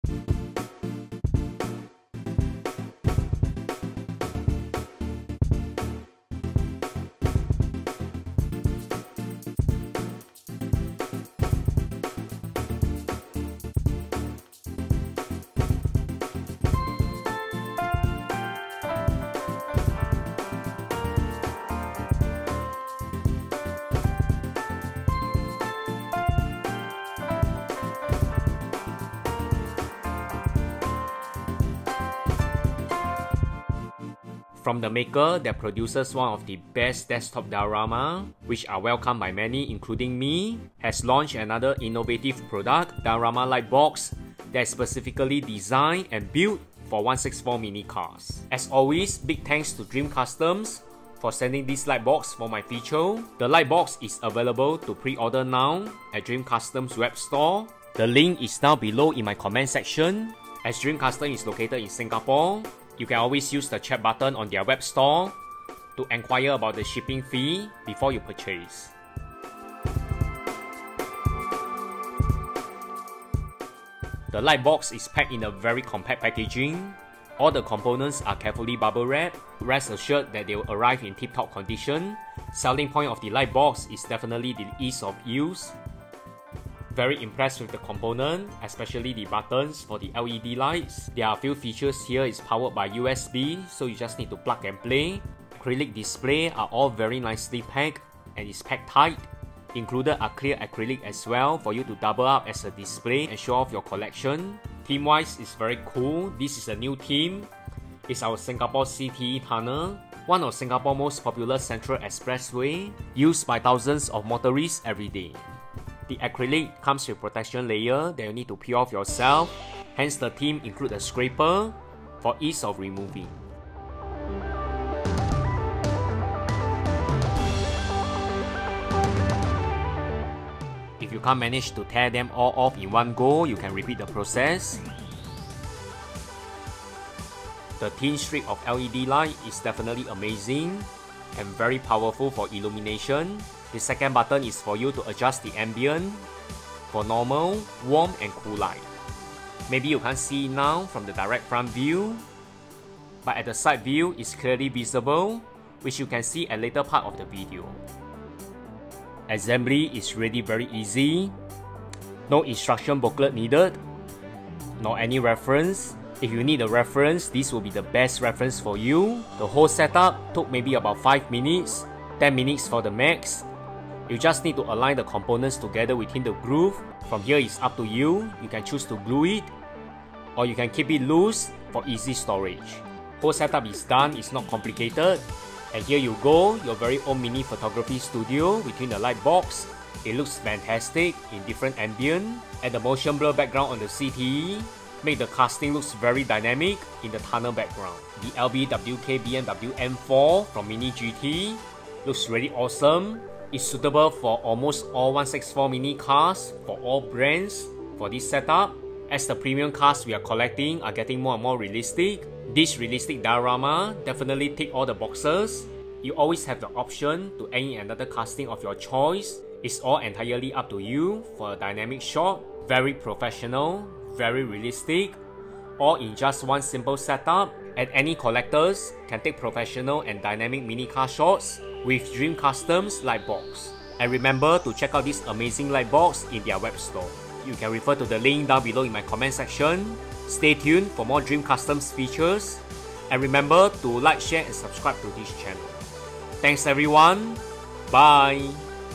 Take Dynamic Mini Car Photos With Dream Customs NEW ハコ Hako Diorama Lightbox | Its EASY
A feature on Dream Customs latest Diorama lightbox built for every collector to take professional and dynamic diorama photography with your trusted camera or phone!

Or
Let’s Play